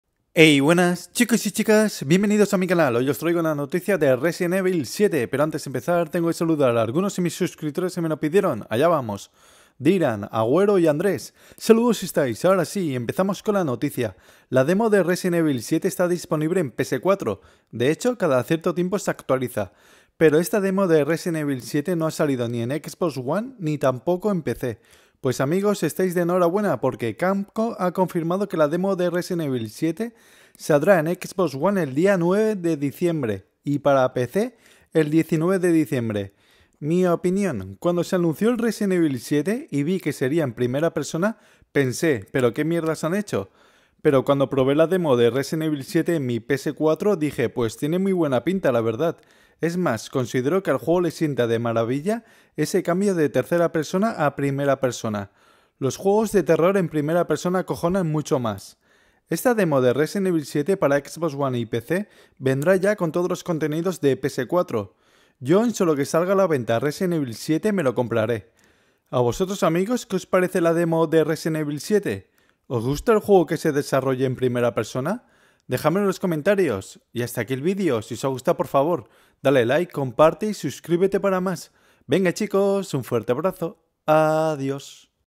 La Demo De Resident Evil 7 Llegara A Xbox One y Pc ( Fecha )
Amigos Podéis Apoyar Al Canal Con Un Donativo Gracias

Mi Twitter: TeteGames HD

Sois Los Mejores Suscriptores Que Un Youtuber Puede Tener